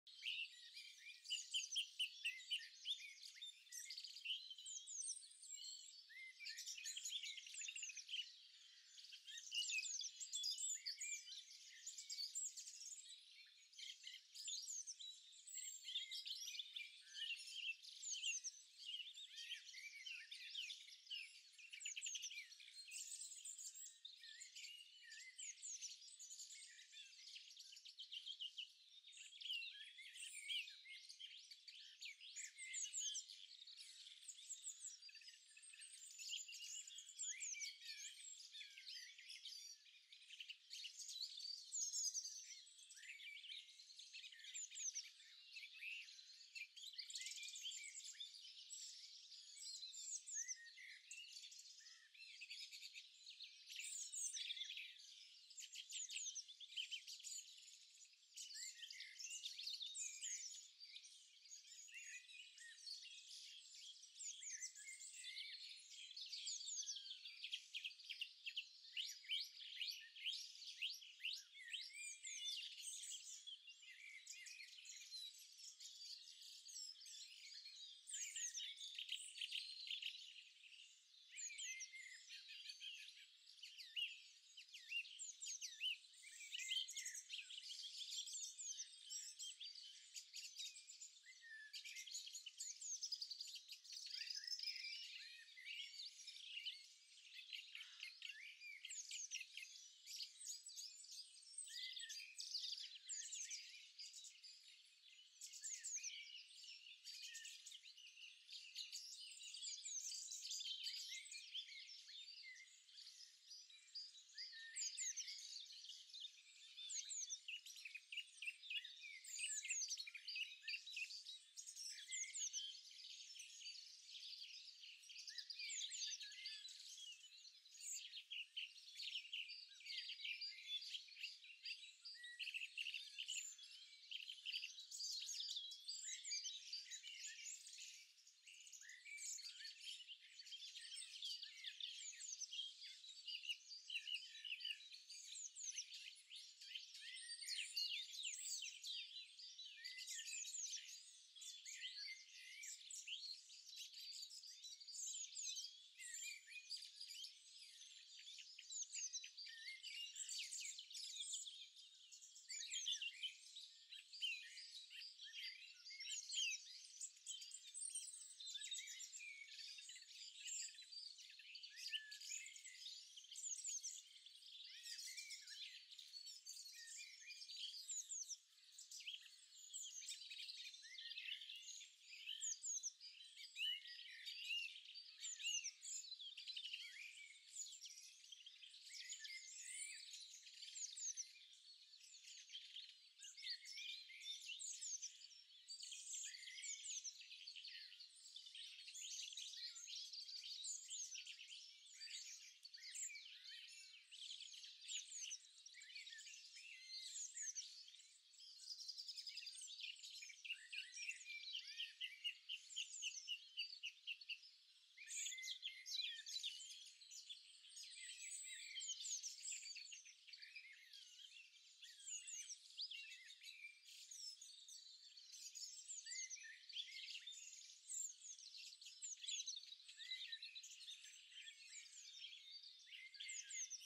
Mollie in Chase Woods
A walk in Chase Woods February 2024